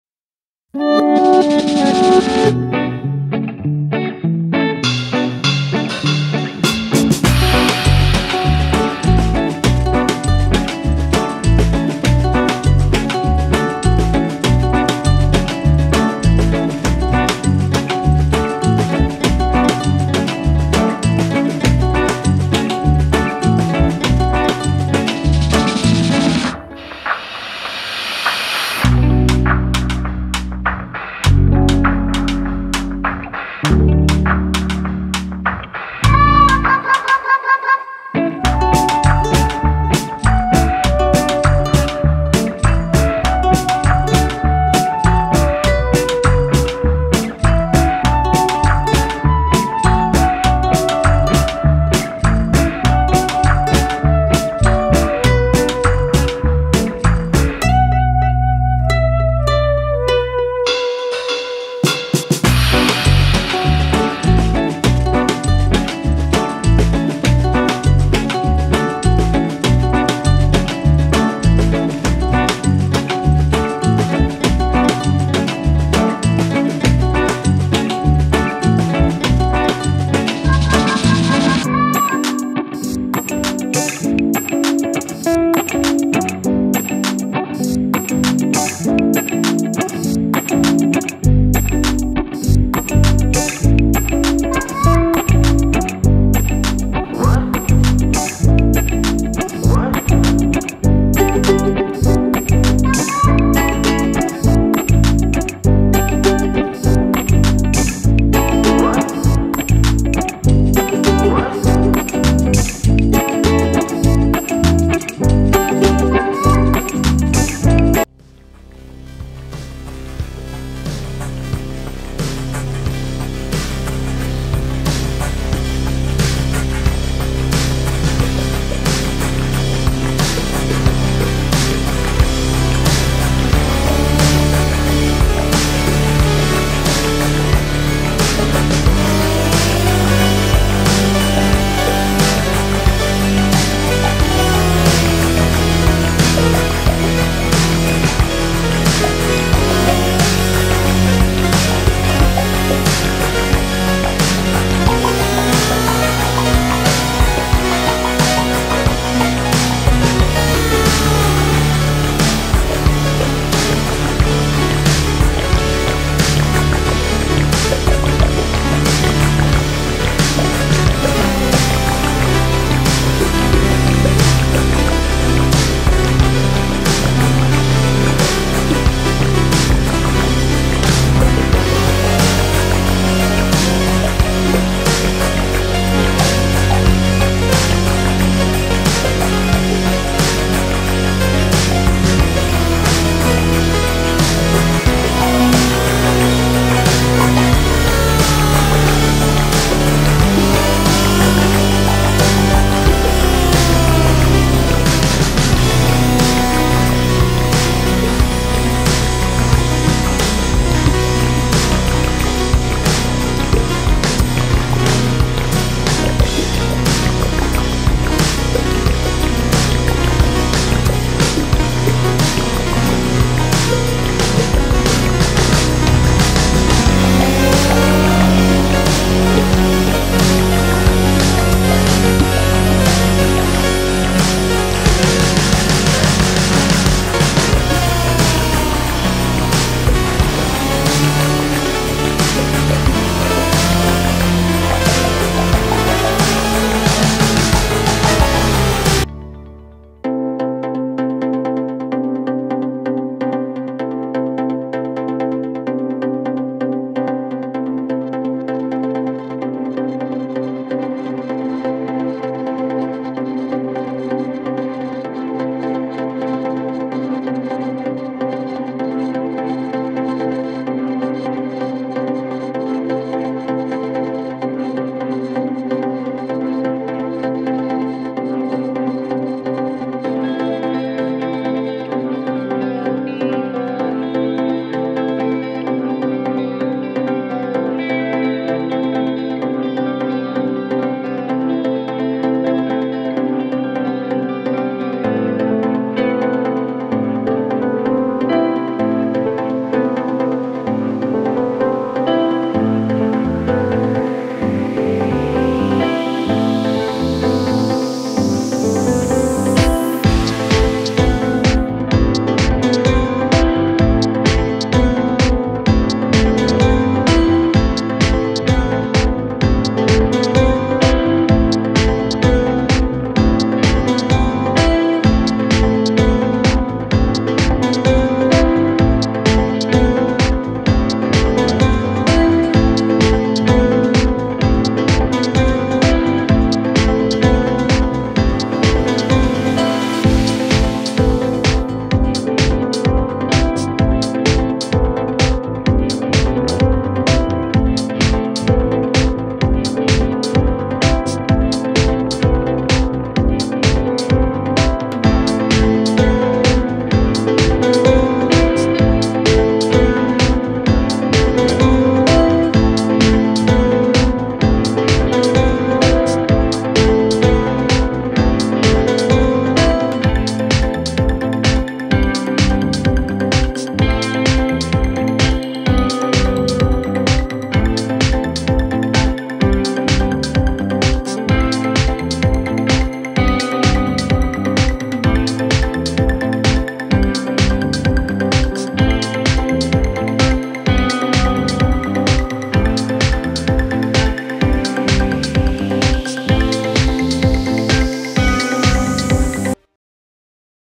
Rainy Season Scenery Drawing for beginners with oil pastel || Girl in rain with Umbrella
Hope you all are doing great. Today I will show you that how to draw rainy season scenery drawing for beginners with oil pastel colours.  I made this drawing with very simple and easy way.  I hope it is very helpful for beginners.

📲SOCIAL MEDIA📲

Things Used:

1. Camel Oil Pastels (50 shades)

2. Camel Oil Pastels (25 shades):

3. Tissue paper:

4. Masking Tape:

5. White Correction Pen:

🎵Music 🎶